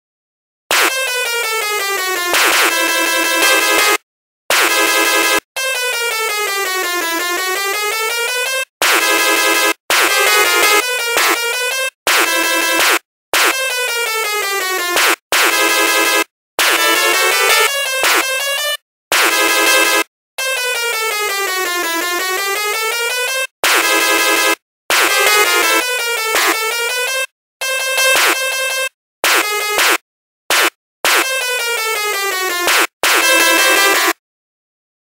Bz
Electric buzz sounds made by me.